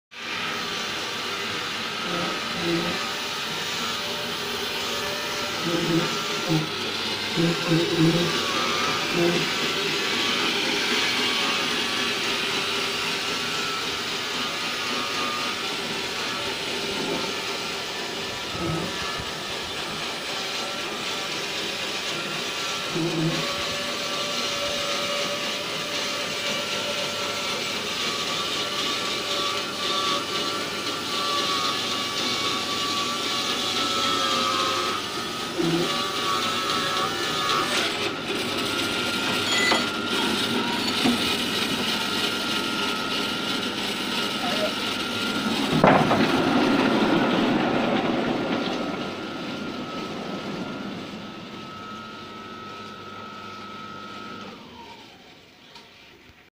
Timber Production at Mapela using Timbery 100 portable sawmill part 3
I enjoyed planting trees when I was young in the early 1990s and now is the time to harvest them after over 26 years! Most people in Western Kenya use power-saw for production of timber and this is very wasteful since most of the wood is wasted as sawdust! As a conservationist, I invested in THIN KERF SAW MILLING TECHNOLOGY and bought a Timbery 100 portable sawmill. The machine is amazing and each day people come to witness how easy it is to turn lumber into timber and other products on a sustainable basis. I also have a sharpening machine to keep my blades sharp!